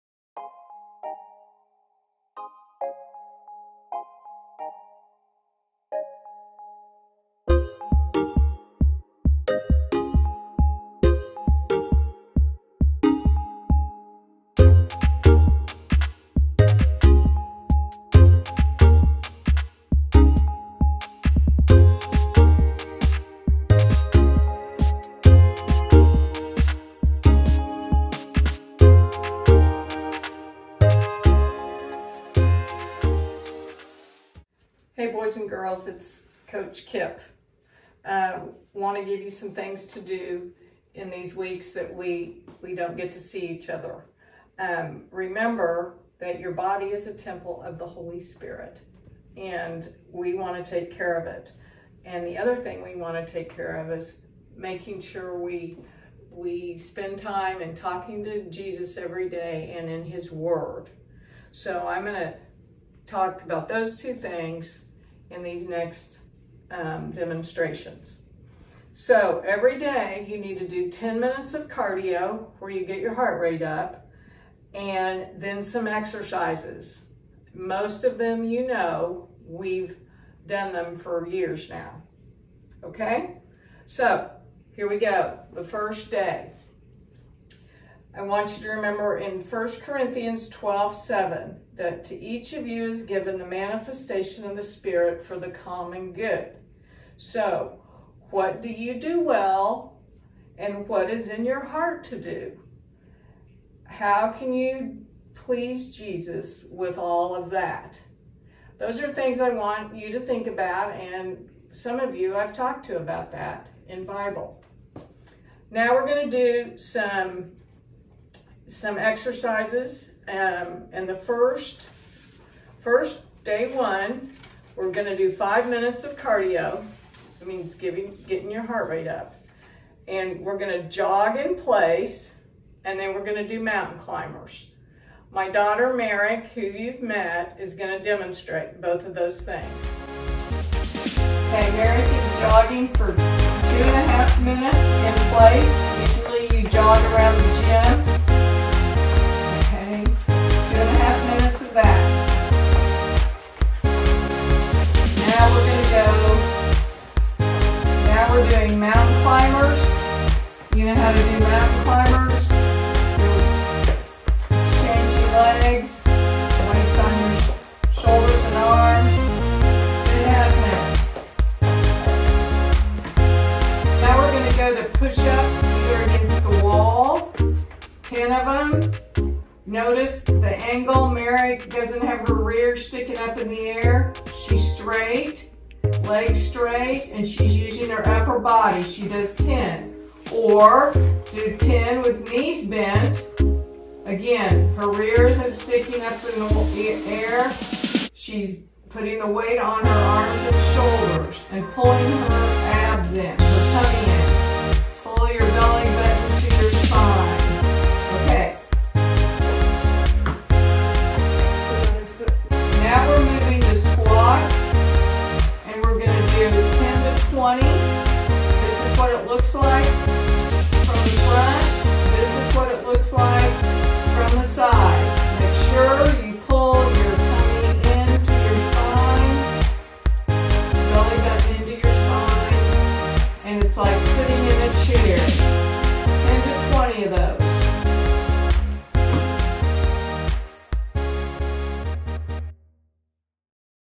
Get ready to MOVE!
Follow along with Coach Cip, as she and her daughter, Merich, teach us how to exercise at home!